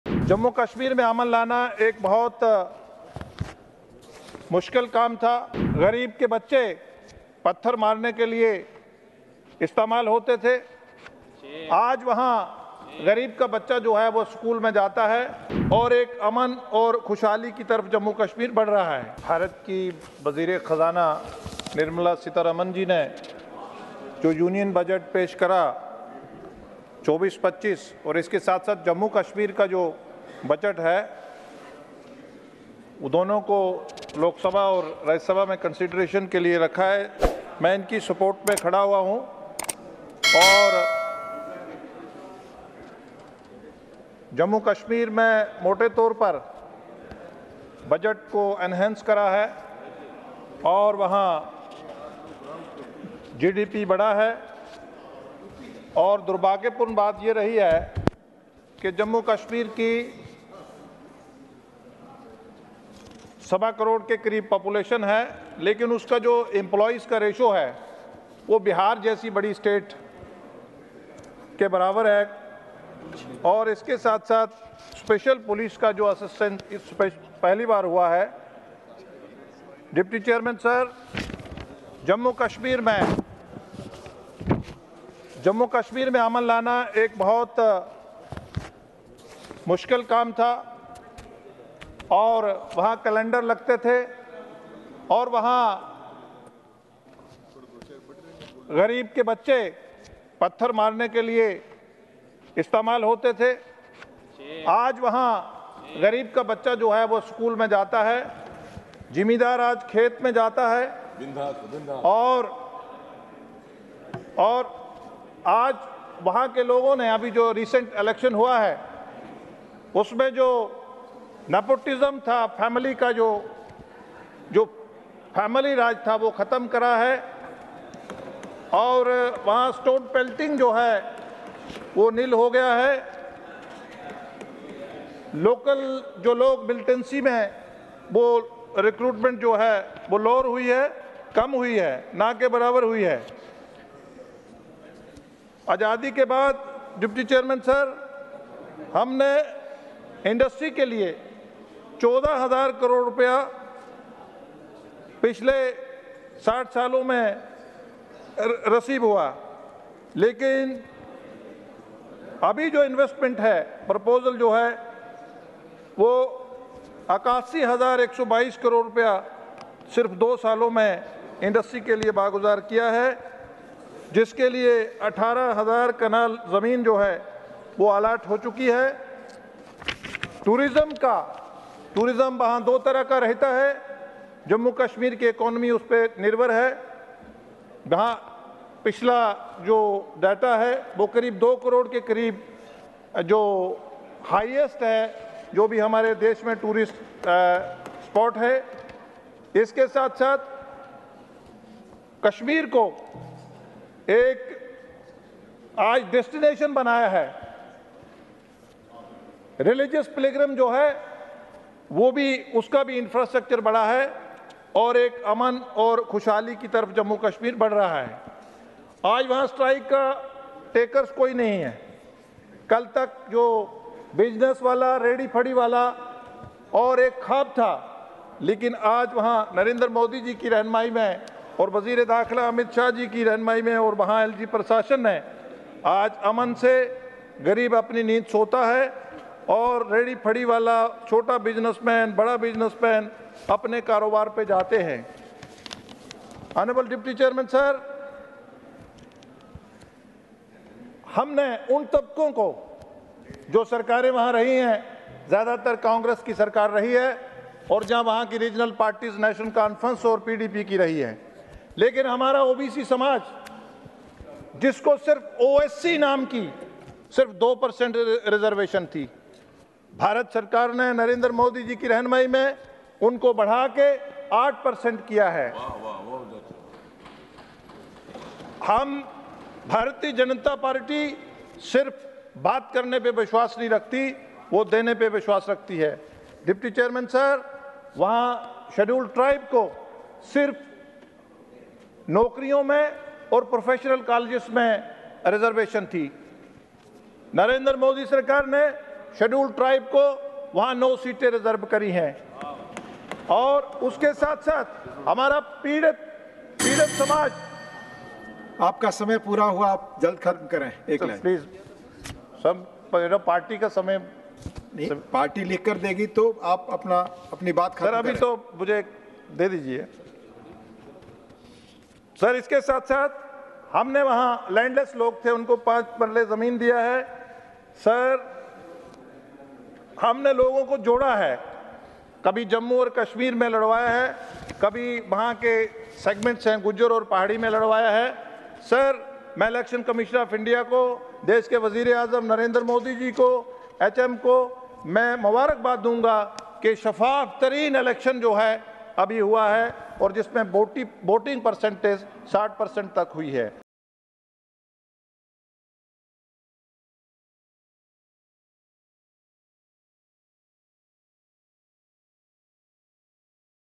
“Jammu Kashmir आज अमन और खुशहाली...” Rajya Sabha में Gulam Ali का जम्मू कश्मीर को लेकर जोरदार भाषण
राज्यसभा में जम्मू कश्मीर के बजट पर समान्य चर्चा के दौरान बीजेपी के राज्यसभा सांसद गुलाम अली ने कहा जम्मू कश्मीर के लिए जो बजट पेश हुआ मैं उसका समर्थन करता हूं। जम्मू कश्मीर में अमन लाना एक मुश्किल काम था। वहां गरीब के बच्चे पत्थर मारने के लिए इस्तेमाल होते थे। आज गरीब के बच्चे स्कूल जाते हैं। किसान आज खेल में जाते हैं। आज जम्मू कश्मीर अमन और खुशहाली की तरफ बढ़ रहा है।